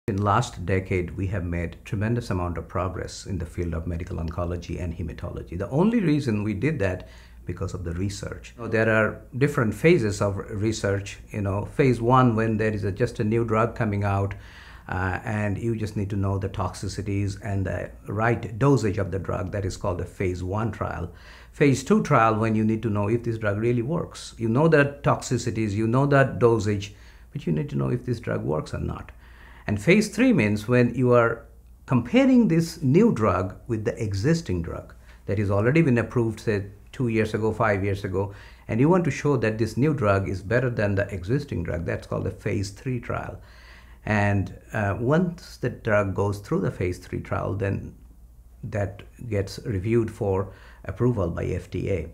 What are some advances that have been made in medical oncology? - Terre Haute Regional Hospital
In the past several years, tremendous strides have been made in the world of medical oncology. A main reason for this is research, especially clinical trials. Ashis Chakrabarti, MD, goes over the different phases of clinical trials and how this process can lead to new medications.